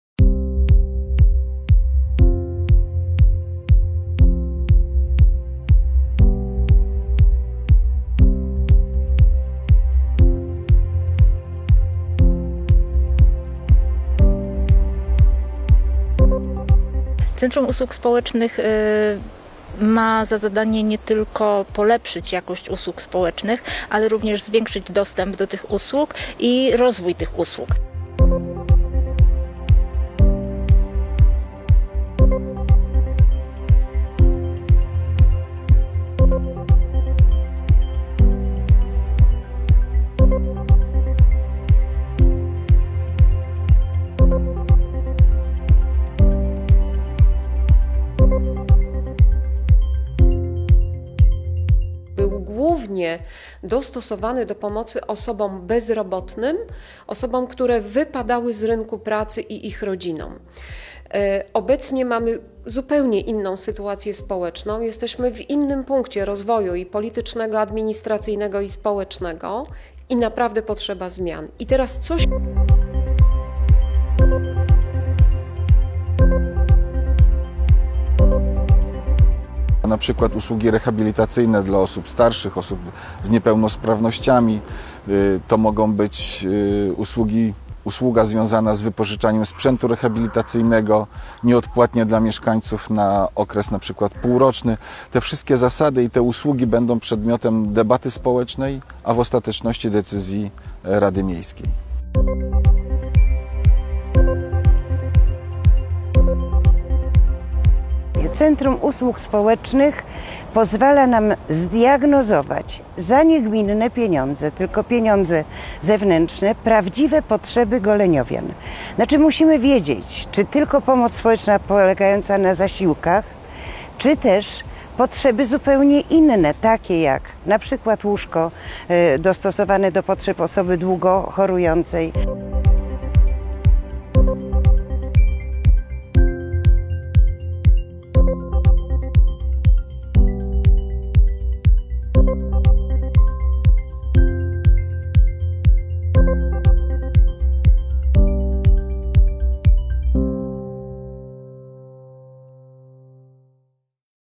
Centrum Usług Społecznych zamiast OPS
Od 1 pażdziernika w Goleniowie działa Centrum Usług Społecznych. Przejęło ono zadania OPS, ale będzie też miało nowe kompetencje. Usługi społeczne mają objąć wszystkich mieszkańców gminy, a nie tylko tych, którzy np. korzystali z zasiłków. To efekt przystąpienia Goleniowa do pilotażowewgo programu oferowania nowowczesnych usług społecznych.